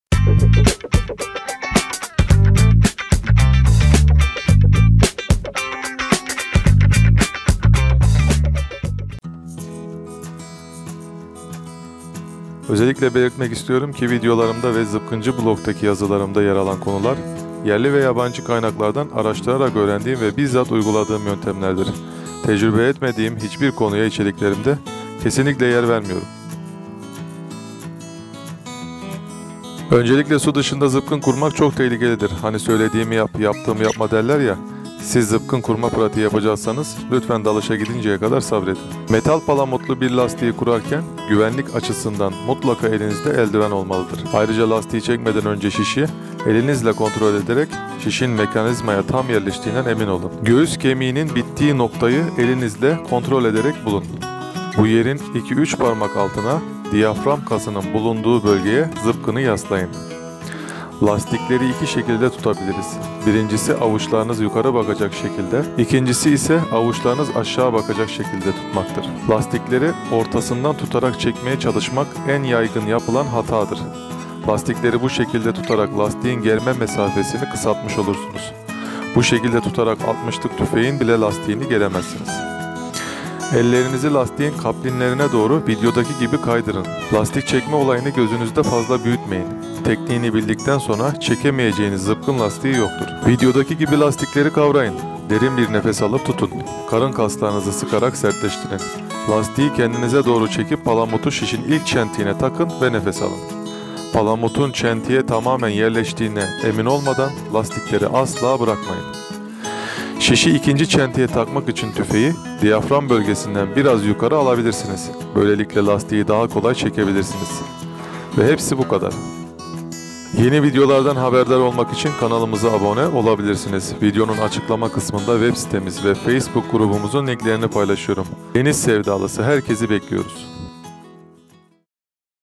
En Kolay Zıpkın Lastiği Çekme Yöntemi | Zıpkınla Balık Avı Nasıl Yapılır ? | Spearfishing Turkey
Zıpkın Avı Türkiye Forum (Serbest Dalış ve Zıpkınla Balık Avı Yapan Herkesi Bekliyorum)

Zıpkın lastiği çekmek, tüfeği kurmak aslında doğru şekilde yapıldığında çok kolay bir şekilde yapılabilmektedir. Zıpkınla balık avına yeni başlayan arkadaşlar için büyük bir problem olan lastik germe konusunu detaylı bir şekilde anlattım. Umarım beğenirsiniz.